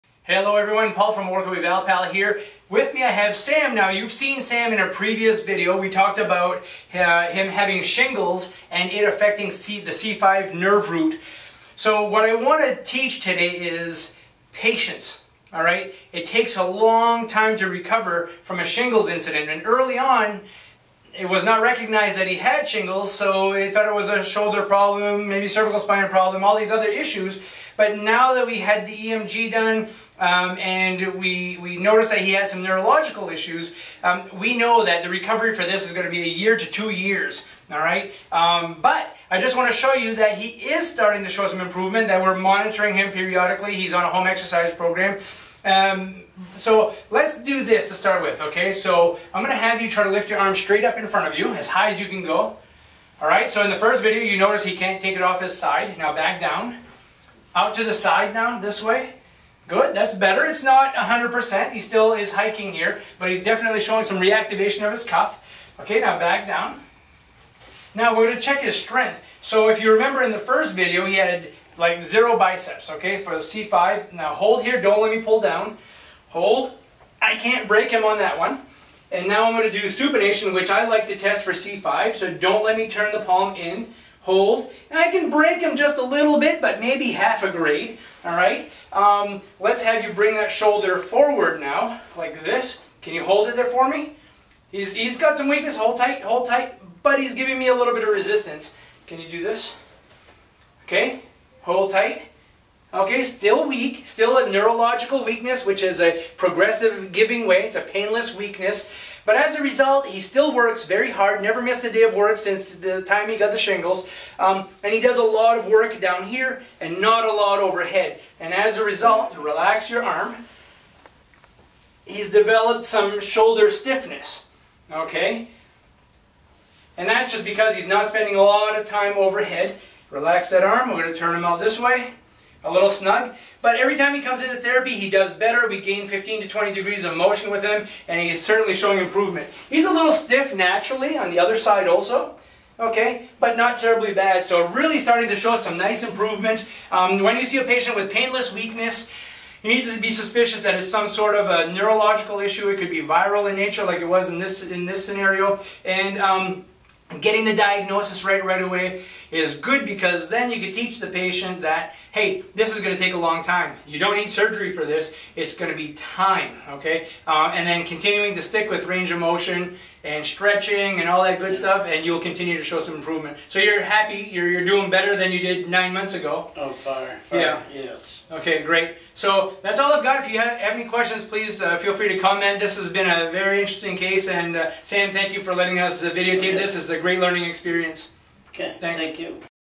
9 months after shingles affecting the C5 nerve root
Check this out! This gentleman had shingles mimicking a C5 nerve root compression. Shingles can also mimick a rotator cuff tear. You don't want to miss this one in the office. Take a look at his improvement after 9 months.